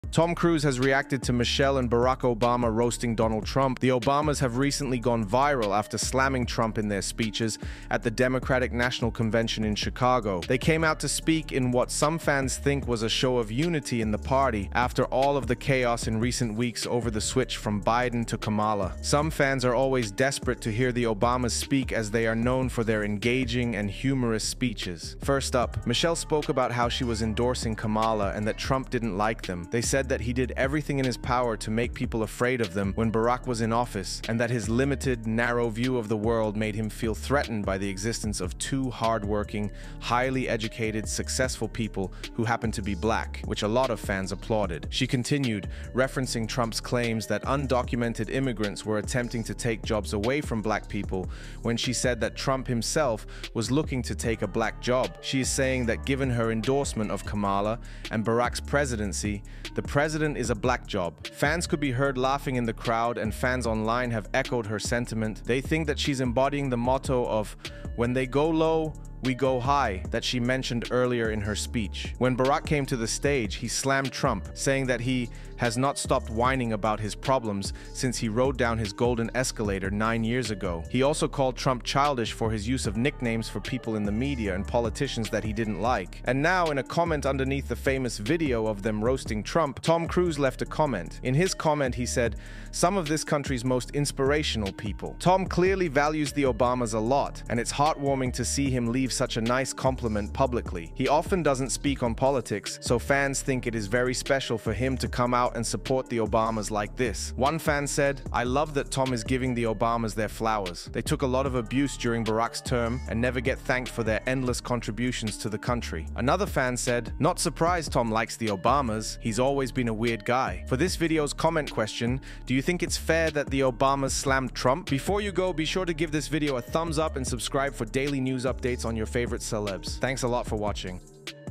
Tom Cruise REACTS to Michelle & Barack Obama Roasting Donald Trump at the DNC
Tom Cruise has reacted to Michelle and Barack Obama roasting Donald Trump at the Democratic National Convention.